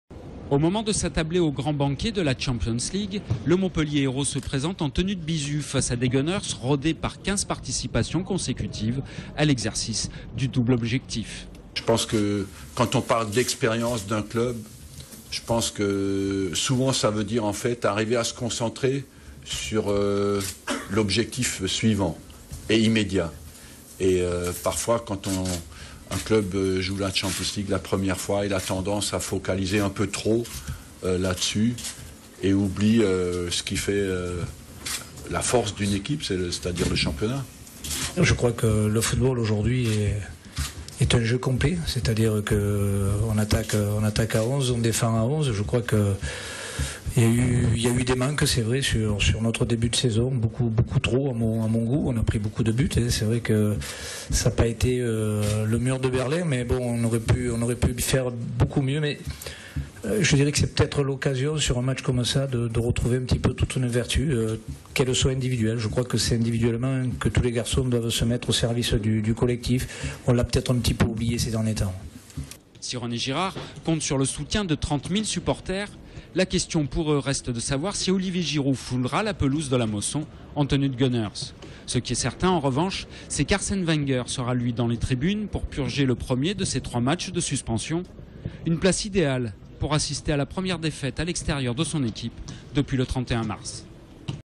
Football : Les attentes de Montpellier et d'Arsenal
Sites webs/blogs, copiez cette vidéo avec le bouton "Intégrer" !
A l'occasion du match historique entre le MHSC et le FC Arsenal lors de la Ligue des Champions à Montpellier, les équipes partagent leurs attentes. Pour exploitation TV, droits réservés, nous contacter